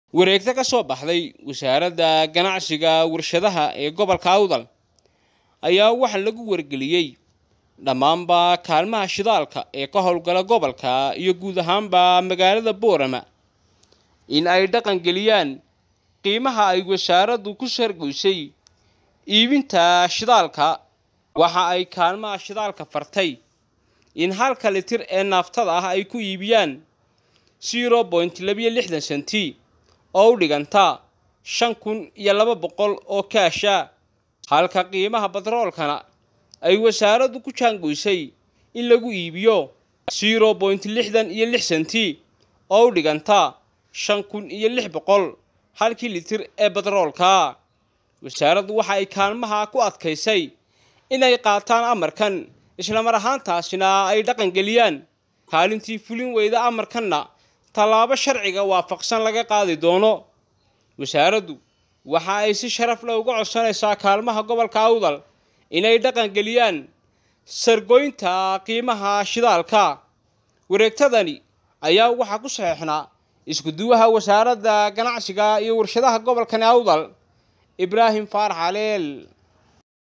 Amar lagu soo rogay kaalmaha shidaalka Boorama ka hawl gala
Amar lagu soo rogay kaalmaha shidaalka gobolka Awdal iyo kuwa Boorama ka hawl gala